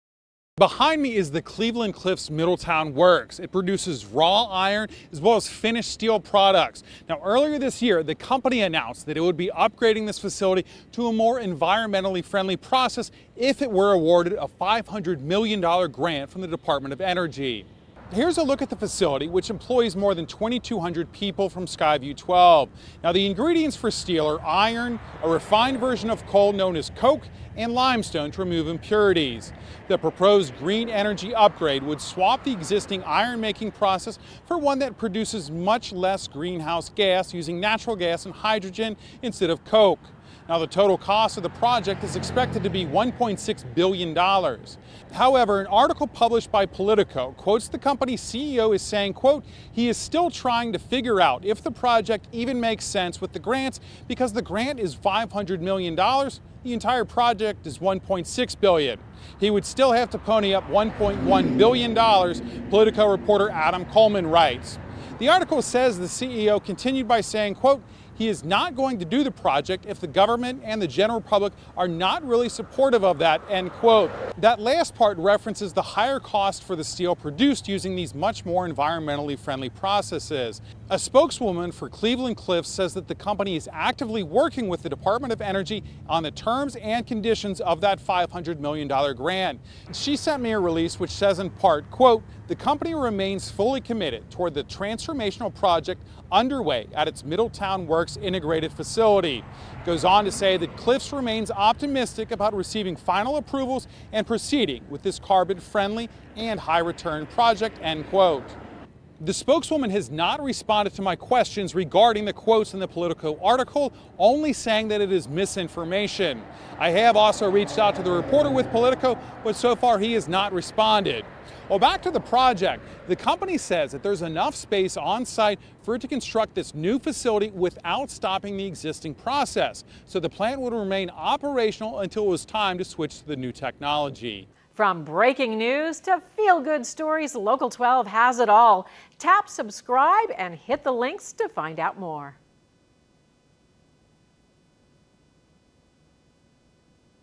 Ohio's $1.6B 'green steel' project still in the works despite previous reports
A few months after announcing a major environmental upgrade at the Cleveland-Cliffs Middletown Works steel mill, the CEO of the company has expressed doubts about the project's feasibility to a reporter with POLITICO